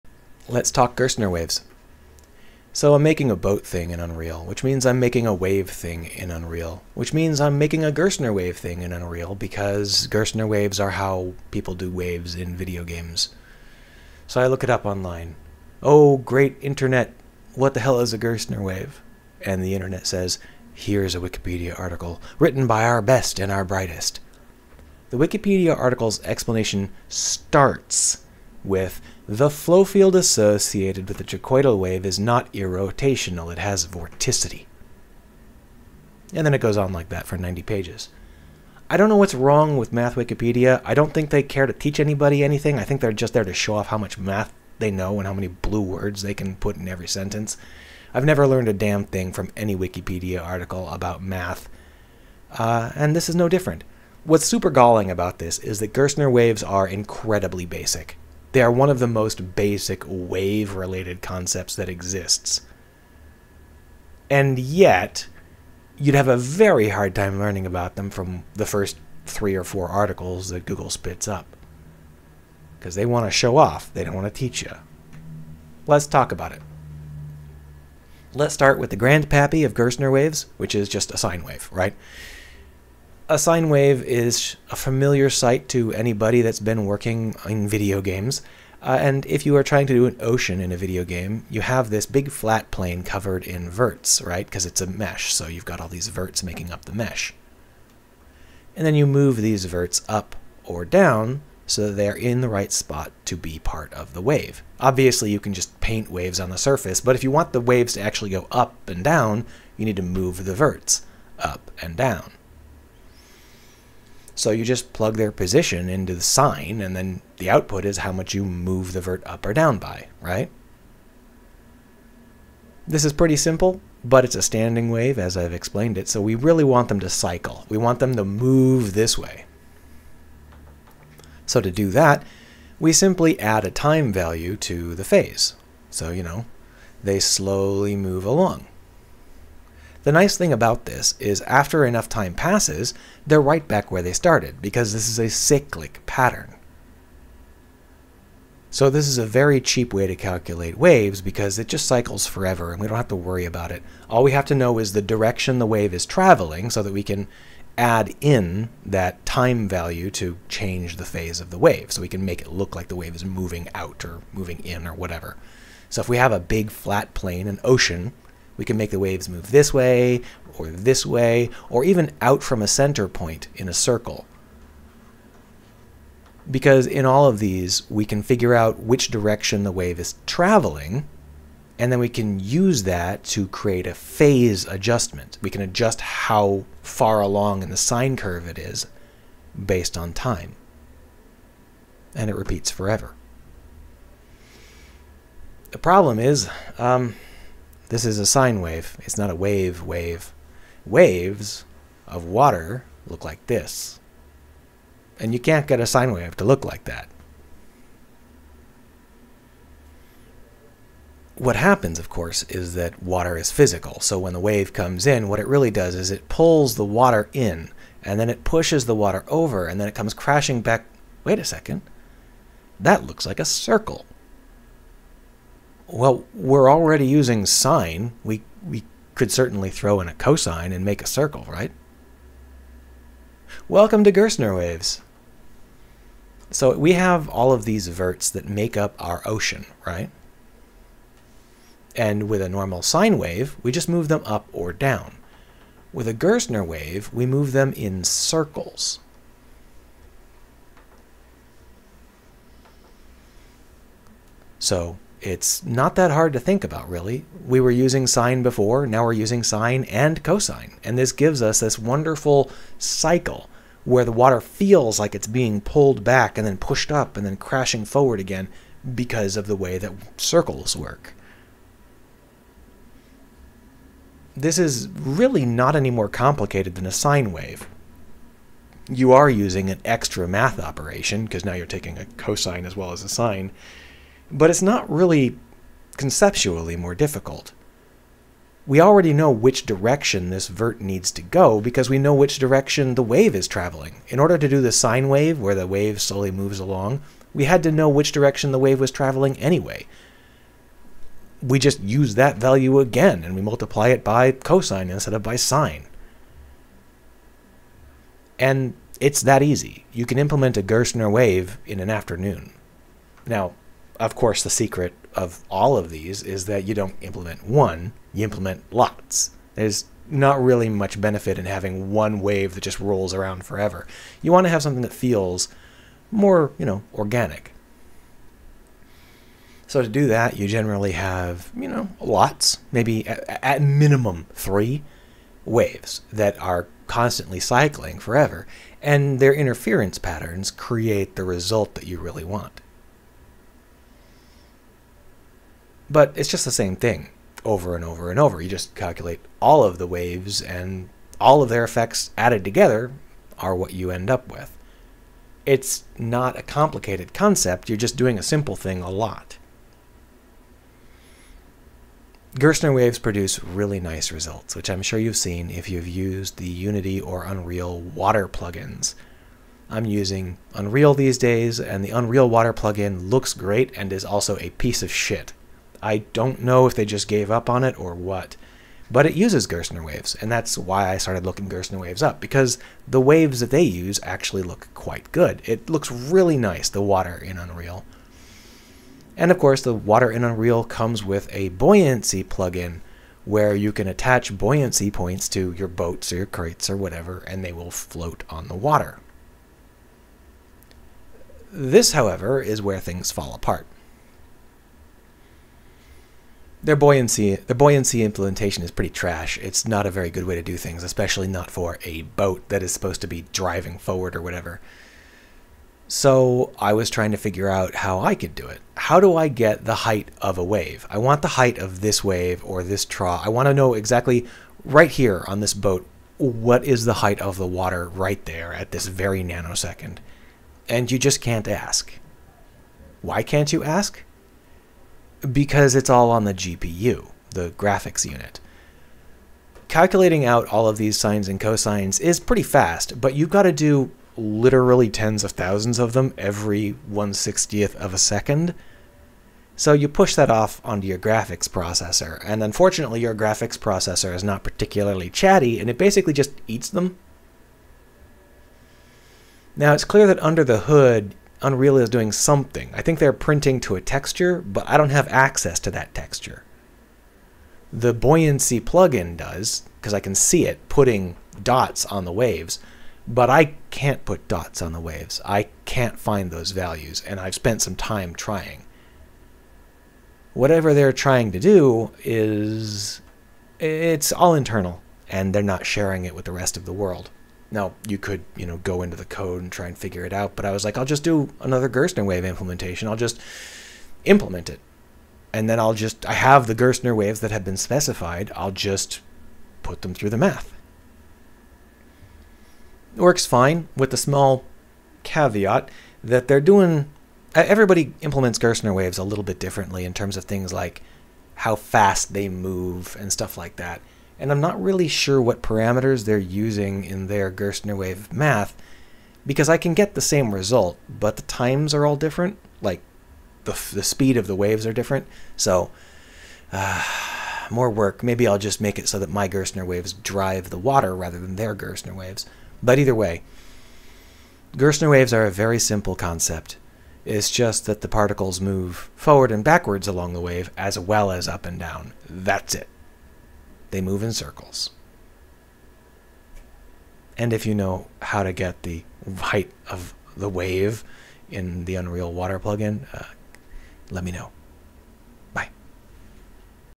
What are Gerstner Waves?
Let's talk about the most badly-explained easy concept I've seen this year.